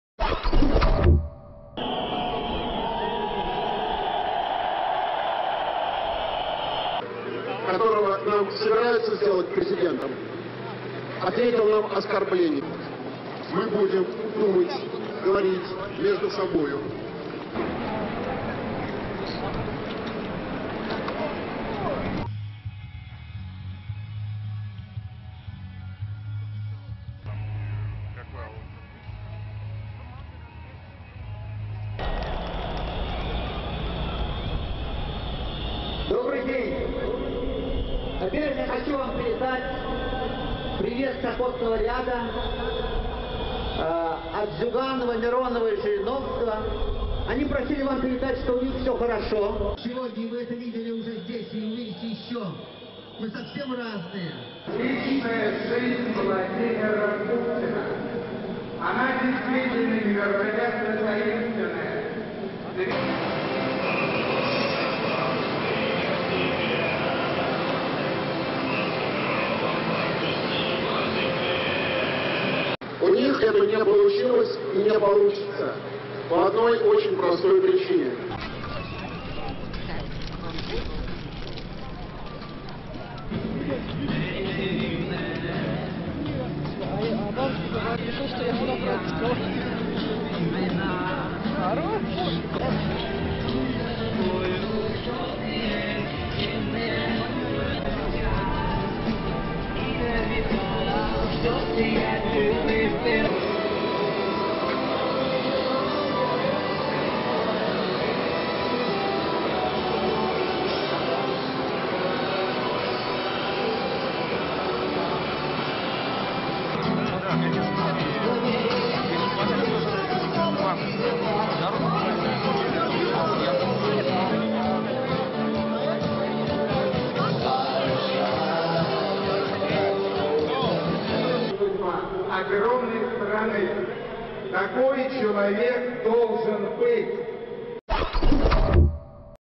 December 24 Fair Elections Sakharov Avenue Moscow Rally
Organizers of last weekend's anti-Kremlin rally in Moscow, the biggest street protest since 1993, were divided on whether they could press authorities into cooperating
Anti-corruption activist Alexei Navalny wrote on his LiveJournal blog that "inhabitants of the Kremlin declared readiness to evolve for the sake of survival."
"For now, this readiness appears to be a ruse and a con game, but that's normal. Everyone wants to save face,"  wrote Sunday.
But liberal opposition leader Garry Kasparov said a new prison sentence for radical protester Sergei Udaltsov, passed Monday, indicated that the Kremlin plans no compromise.

"I think what people in Russia want are the kind of things that people in the U.S. and Western Europe take for granted," says Luke Harding, award-winning foreign correspondent with The Guardian of London. "They just want fair elections. They want a real, clearer media that listens to opposition voices, that has critical people who are currently banned from state television. They want a more plural political landscape," Harding was expelled from Moscow earlier this year after he used classified diplomatic cables published by WikiLeaks to report on allegations that Russia, under the rule of Putin, had become a "virtual mafia state." Harding has written a new book about his experience, "Mafia State: How one reporter became an enemy of the brutal new Russia."